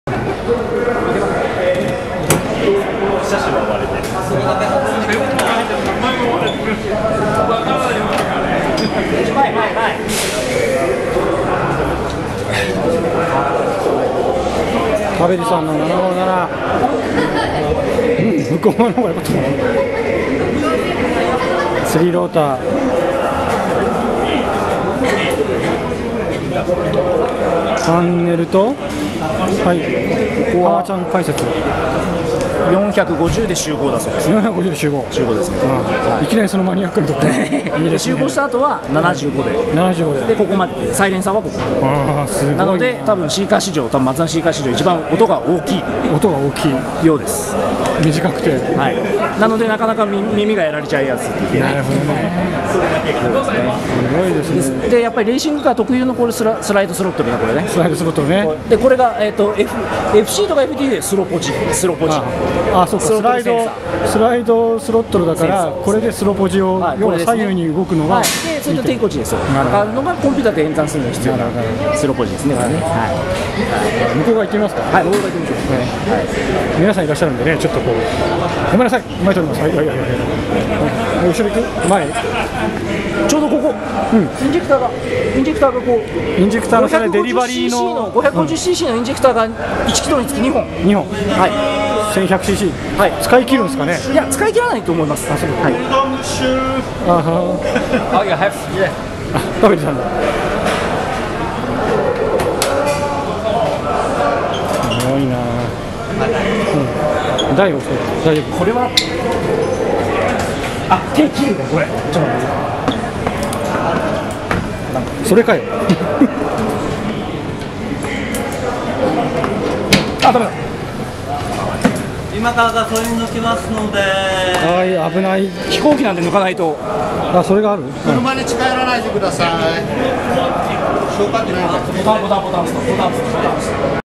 【R.R.F.M.】レーシングロータリーファンミーディング ハマちゃんTalk 2
【R.R.F.M.】限定公開
レーシングロータリーファンミーディングのマツダ757解説　その2